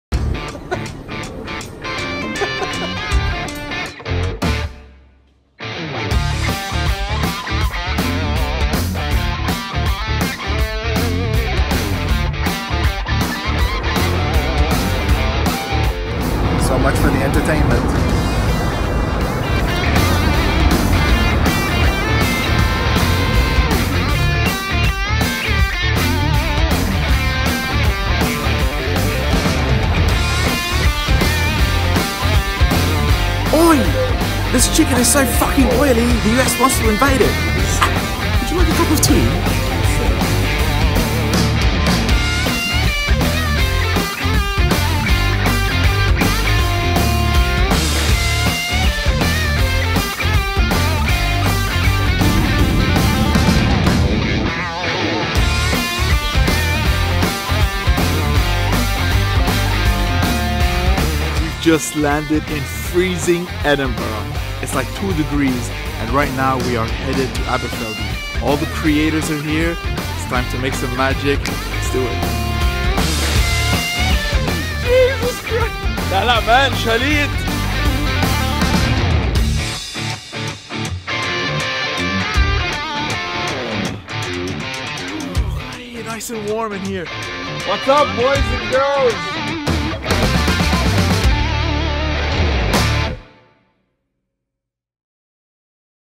Daddy Visits the Home of Dewar's - Ep 1
I'm so pleased to present Episode 1 of my latest collaboration with the the wonderful people at Dewar's

Join me in my journey to Aberfeldy, the Home of Dewar's, as I learn all about the distillation, maturation and appreciation of Dewar's whisky, including a lot of priceless candid moments and unforgettable experiences

The entire experience was shared, captured and immortalized by my main man Stevoo Rizk

Enjoy boys and girls ;)